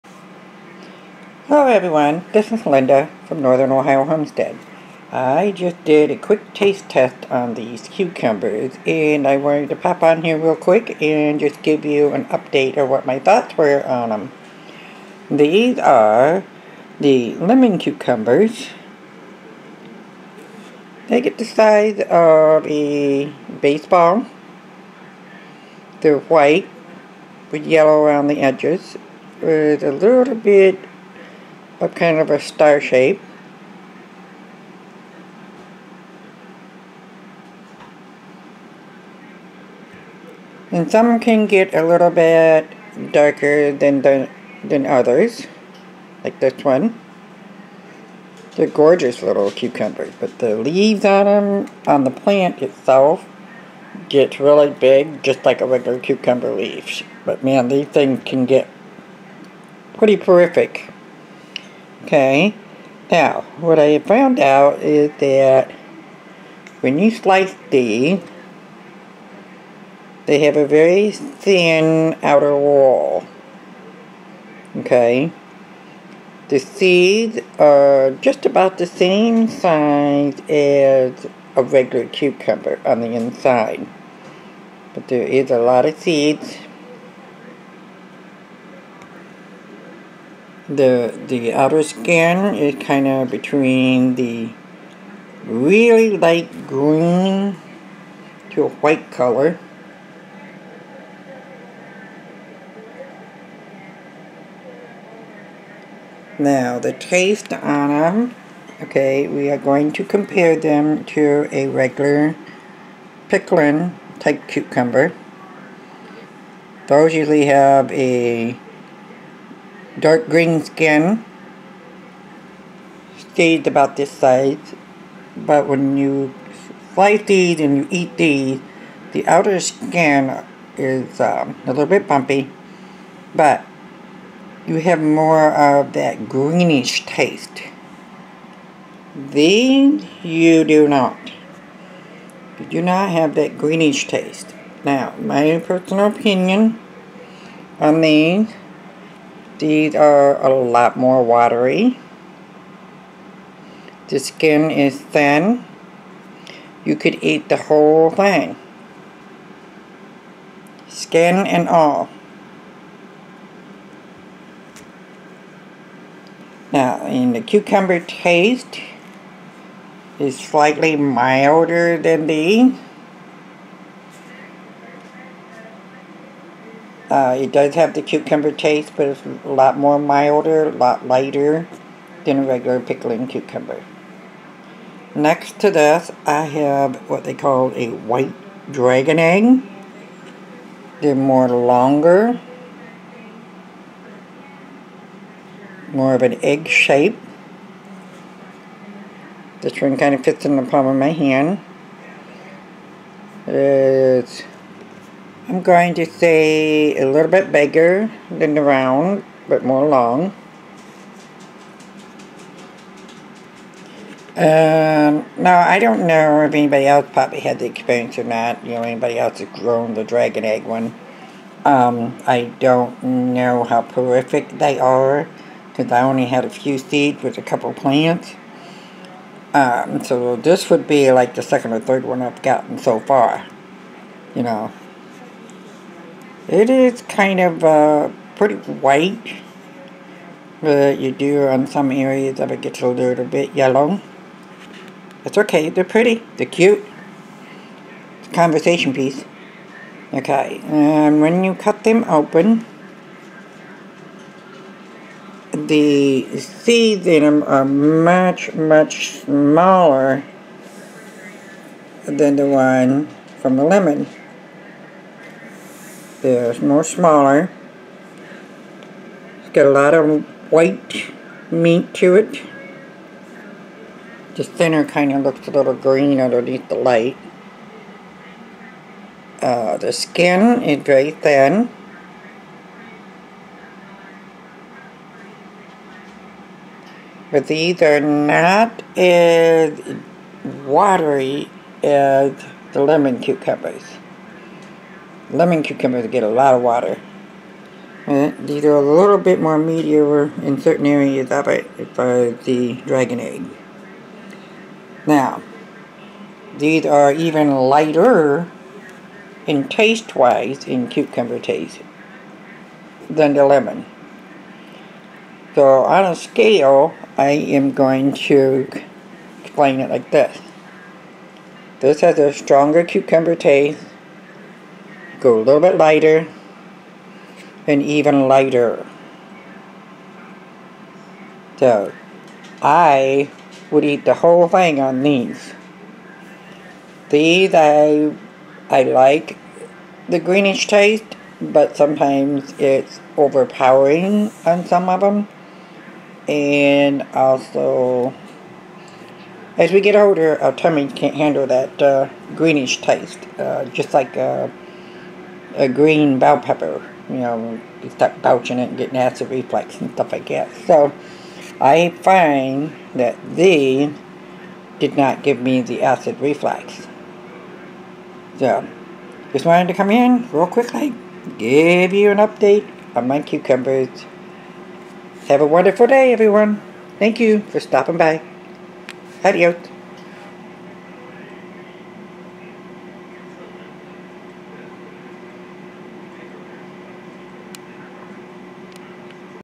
Cucumber Taste Test - Yummy!
Cucumber Taste Test Update. Lemon Cucumber - Dragon Egg. Seeds from Baker Creek Seeds.

Amazon Wish list: